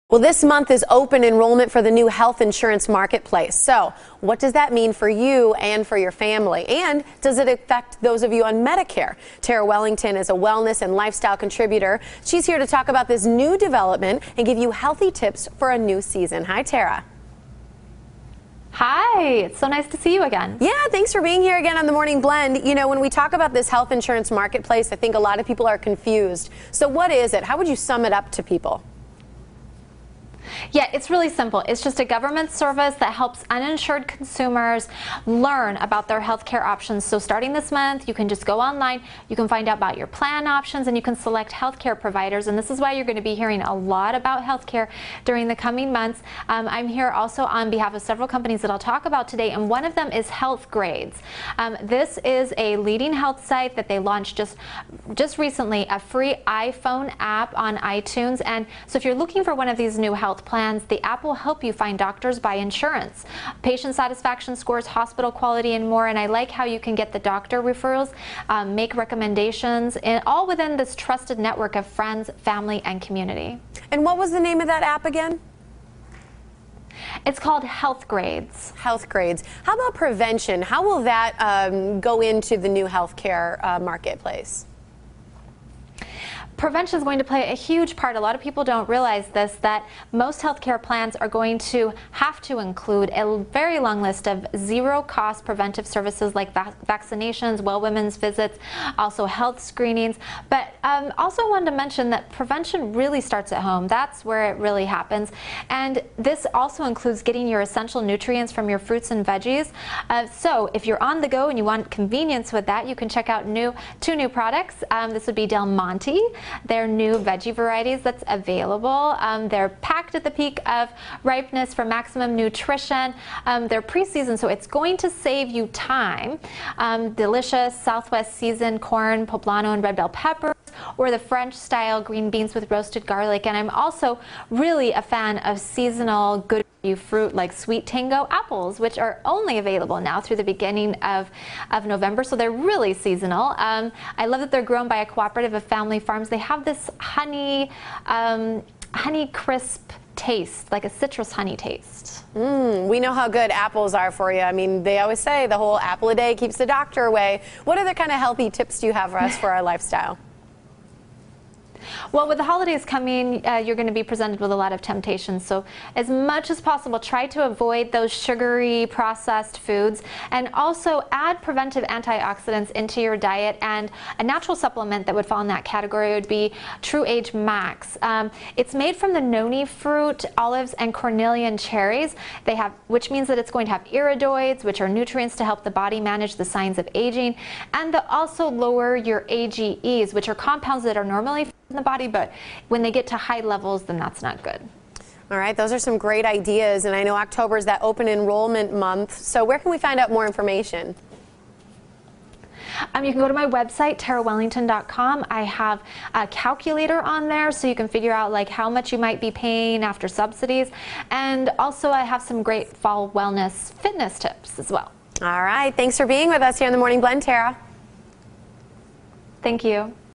Fall Wellness Check-Up
Wellness and lifestyle author Terra Wellington joins us to discuss the changes in healthcare surrounding the Affordable Care Act as well as health tips to keep you and your loved ones flu free this fall.